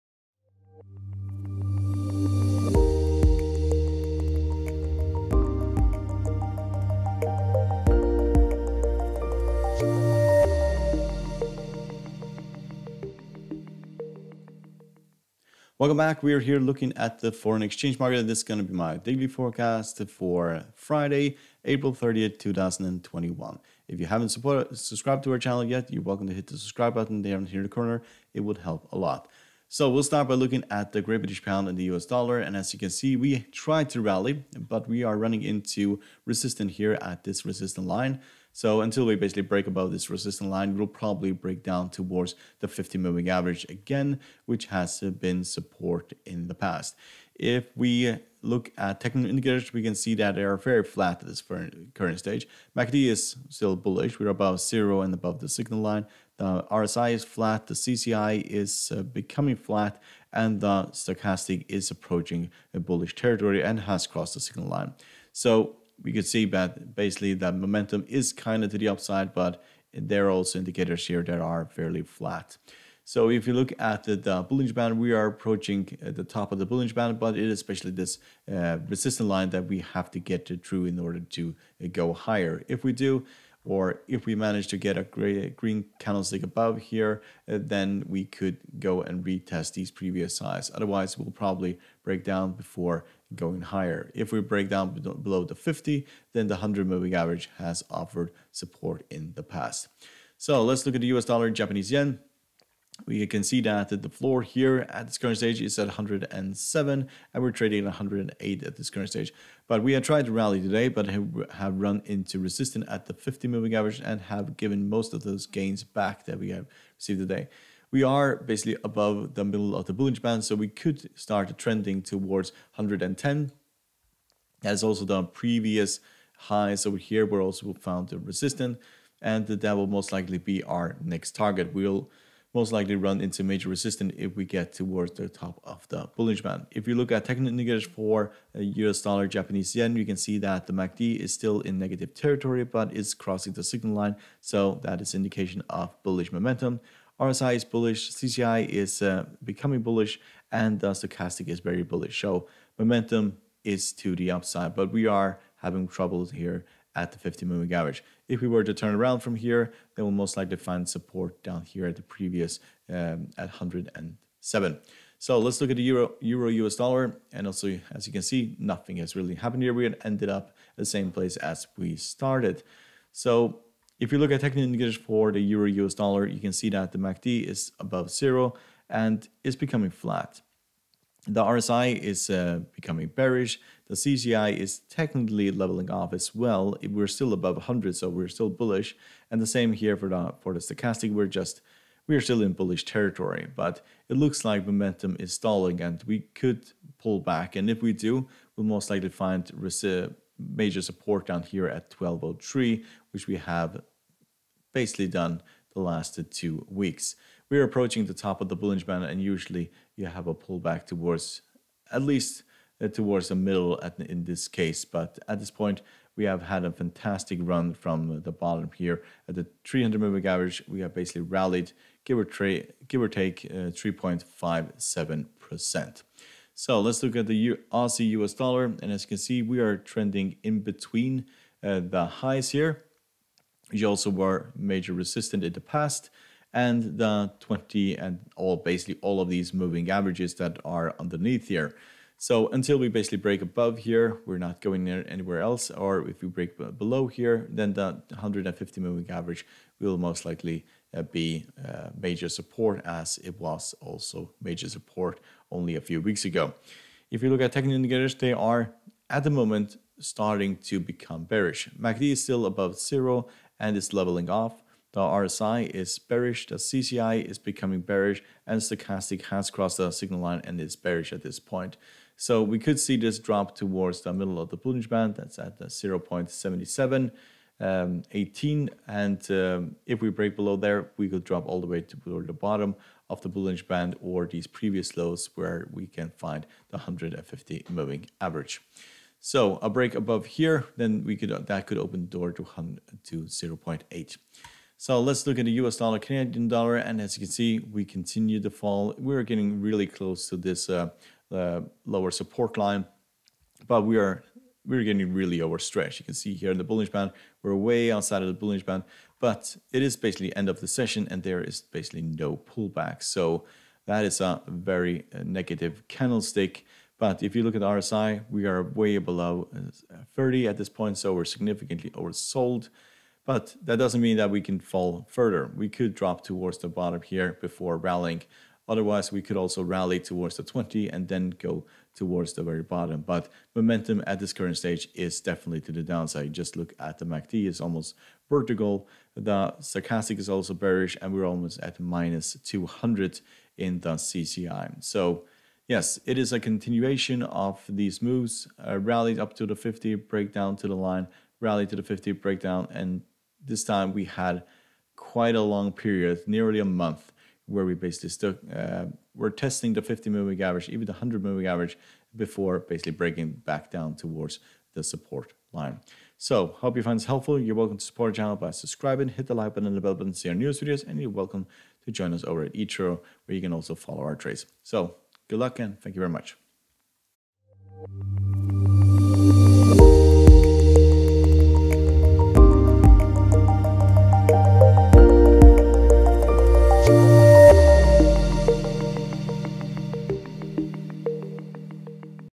EUR/USD, GBP/USD, AUD/USD, USD/JPY and USD/CAD forecast for April 30, 2021
Timecodes
0:00 - Intro
0.14 - GBP/USD
1:41 - USD/JPY
2:46 - EUR/USD
3:43 - AUD/USD
4:54 - USD/CAD
6:13 - End